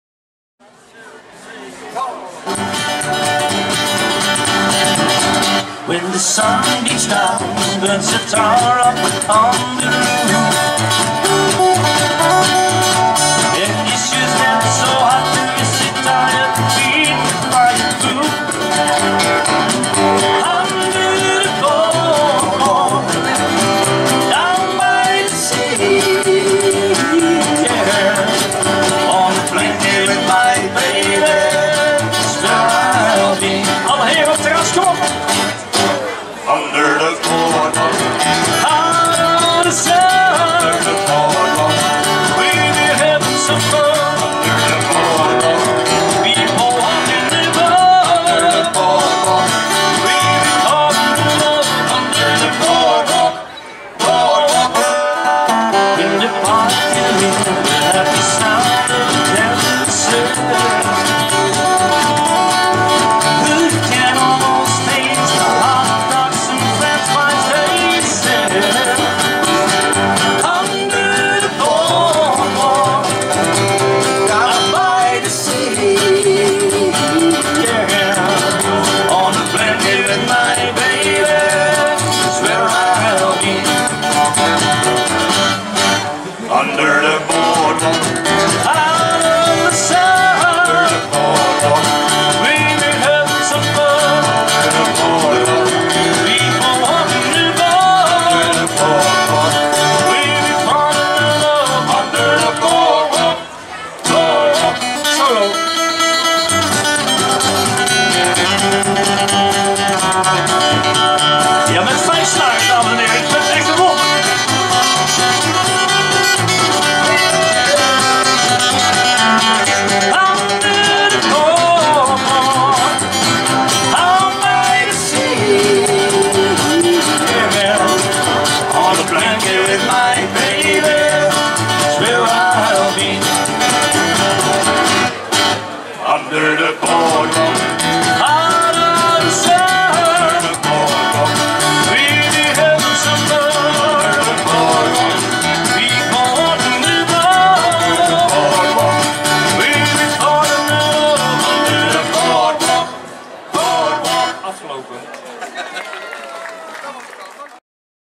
Koningsdag Geleen 2018: The Groovy Gents
Opgenomen bij Café De Kroon op Koningsdag in Geleen.